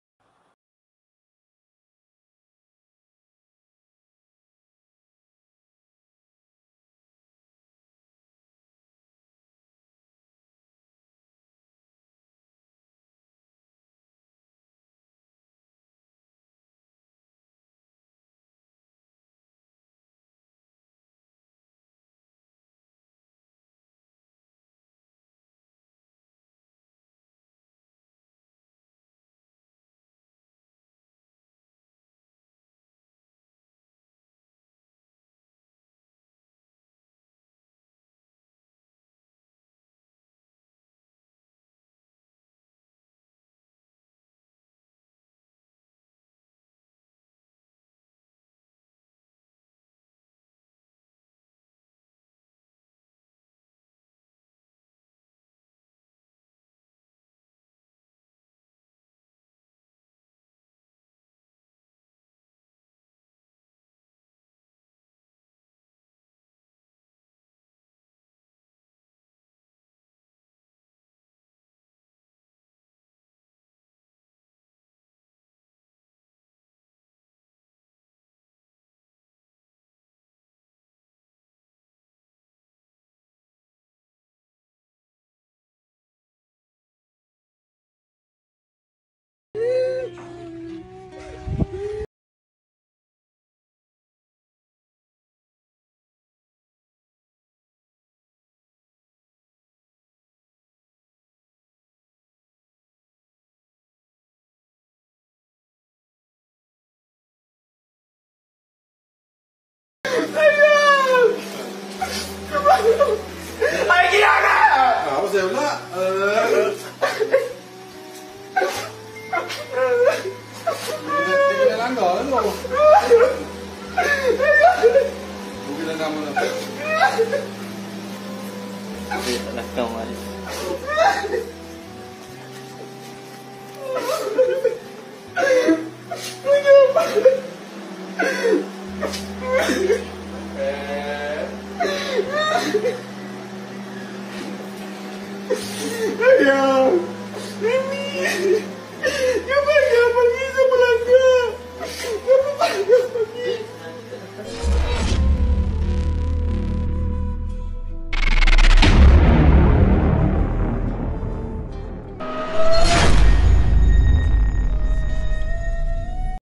Menyentuh Perasaan Apabila Kucing Kesayangan Mati
aksi sedih saat haiwan keaayangan mati membuaykan ramai yang merasa sebak...